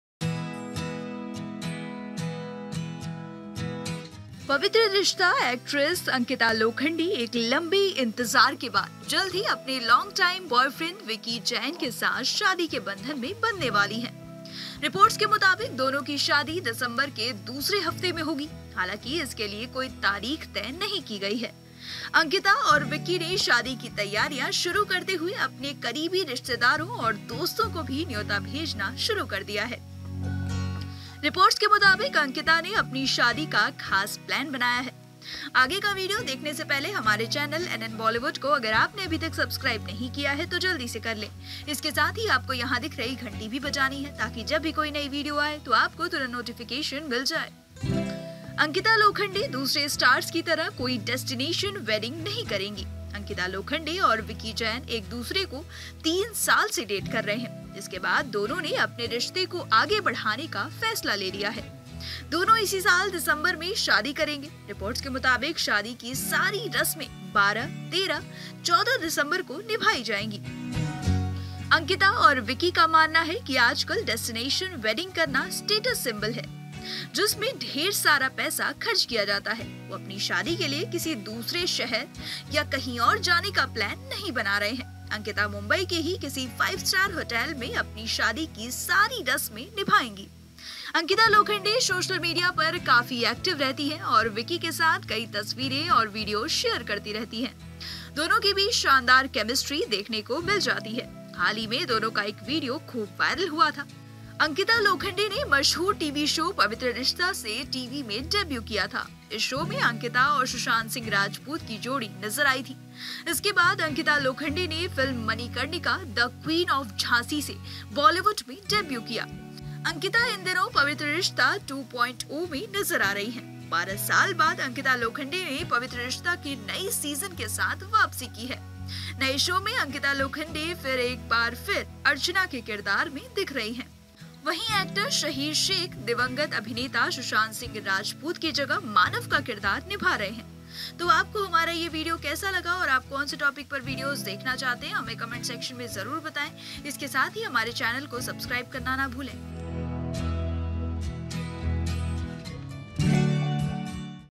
इस दिन विक्की जैन संग 'पवित्र रिश्ते' में बंधेंगी Ankita Lokhande
पवित्र रिश्ता एक्ट्रेस अंकिता लोखंडे (Ankita Lokhande) एक लंबे इंतजार के बाद जल्द ही अपने लॉन्ग टाइम बॉयफ्रेंड विक्की जैन से साथ शादी के बंधन में बंधने वाली हैं. रिपोर्ट्स के मुताबिक, दोनों की शादी दिसंबर के दूसरे हफ्ते में होगी, हालांकि इसके लिए कोई तारीख तय नहीं की गई है. अंकिता और विक्की ने शादी की तैयारियां शुरू करते हुए अपने करीबी रिश्तेदारों और दोस्तों को भी न्यौता भेजना शुरू कर दिया है. रिपोर्ट्स के मुताबिक अंकिता ने अपनी शादी का खास प्लान बनाया है.